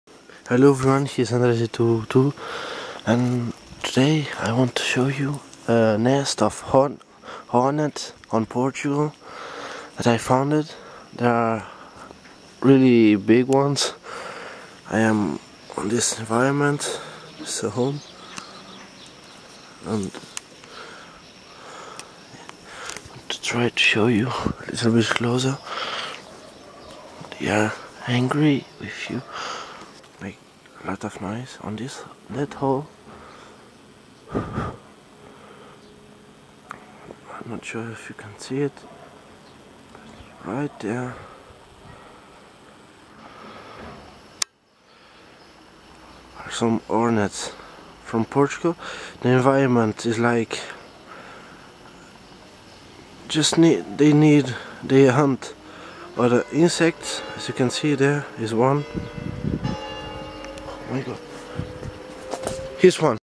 Insect Exploring on Portugal! ( wasp!) Episode 2 (Part 1)
Name of the insect:  wasp is  3 to 4 cm big!